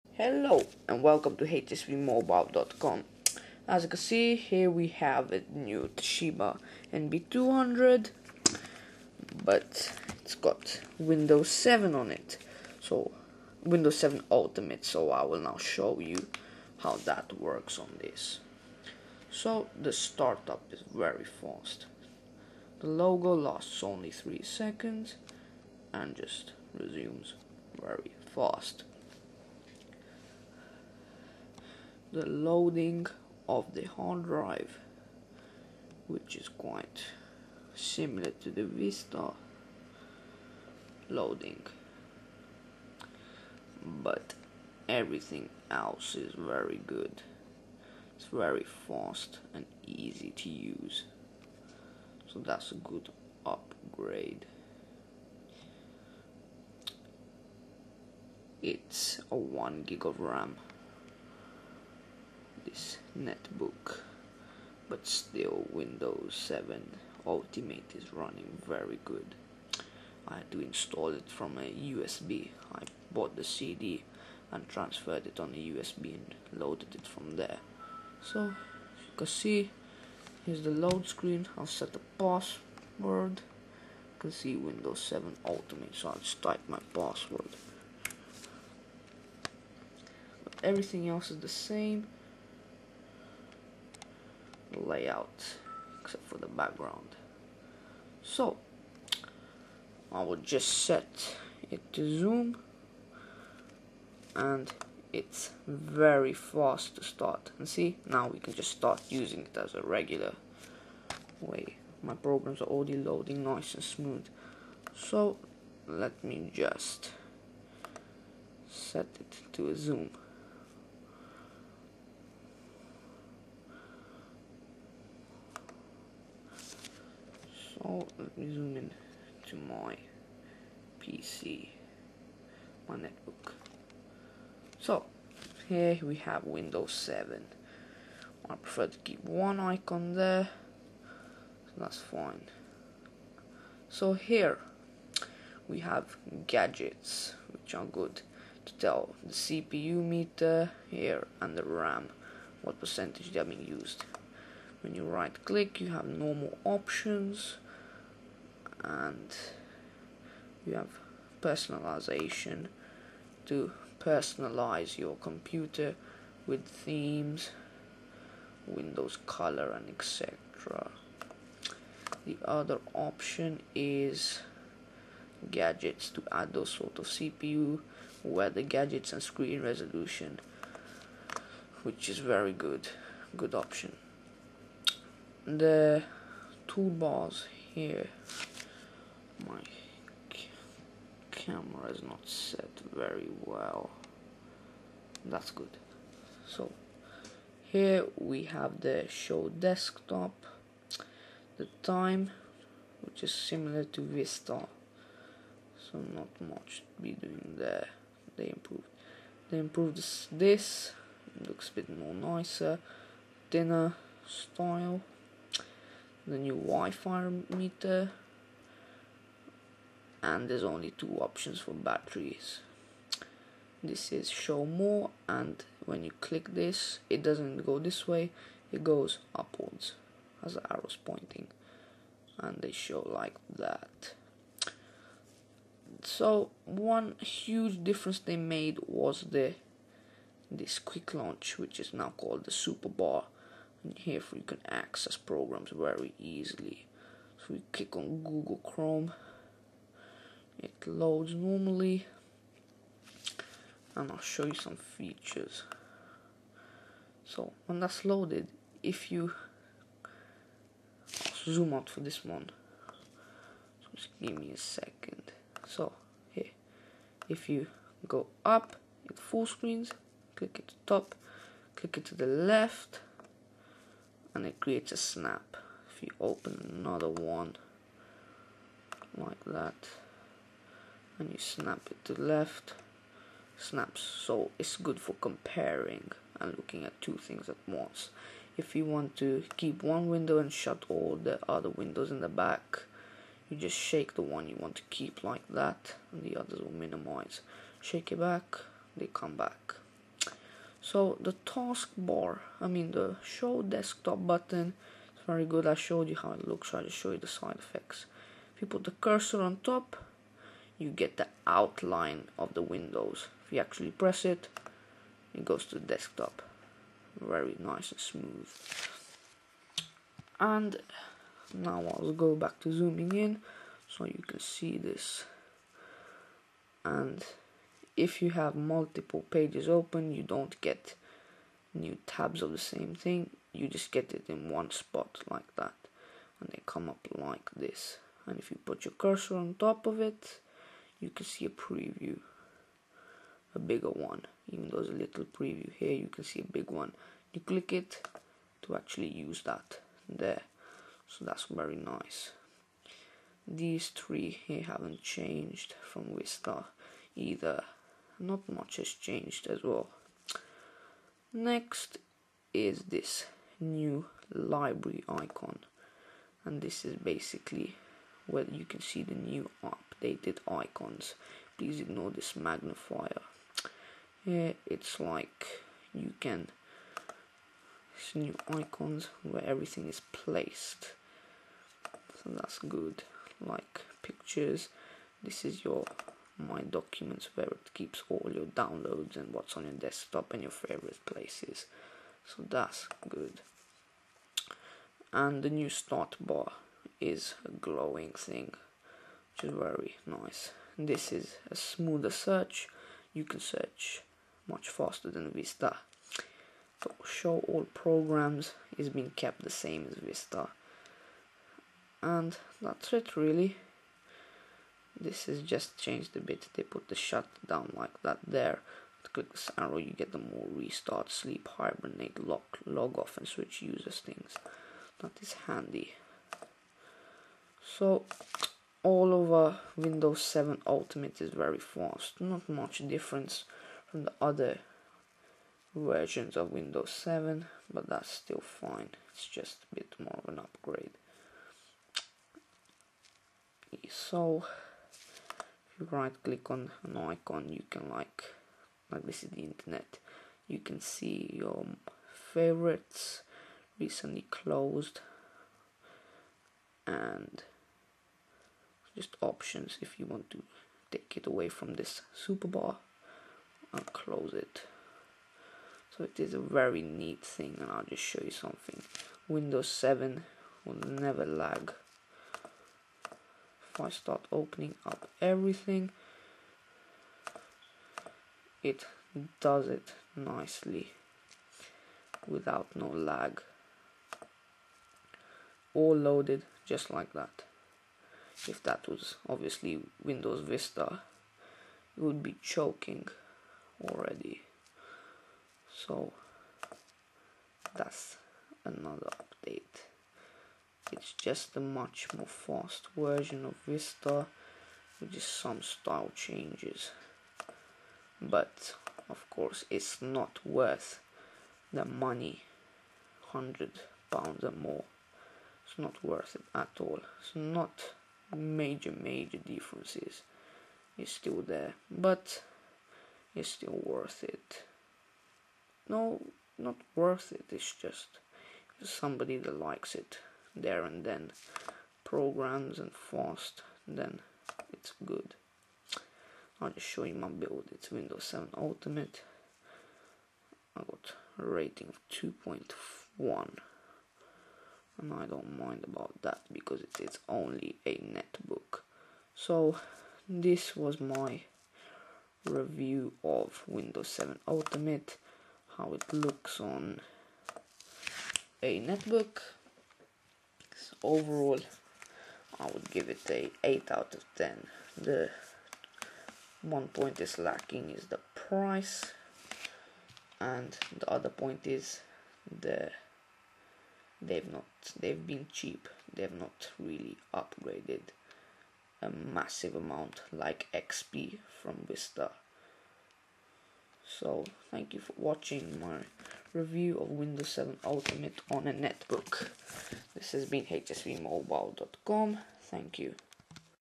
Windows 7 Ultimate on Toshiba NB200 Netbook
checking out windows 7 ultimate on a nb200 netbook